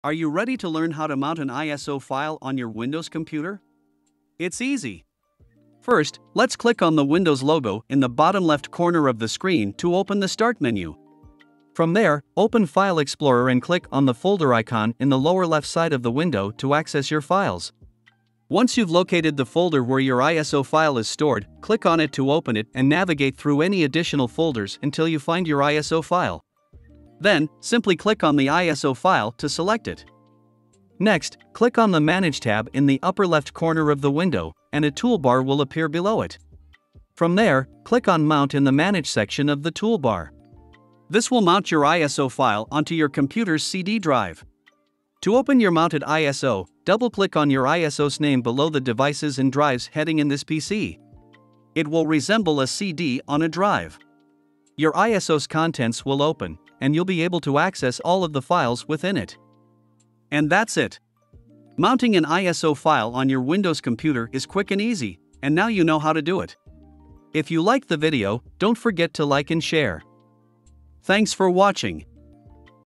How to Mount an ISO File (2026)
Learn how to mount an iso file. It is really easy to do and learn to do it in just a few minutes by following this super helpful tutorial to get a better understanding of everything.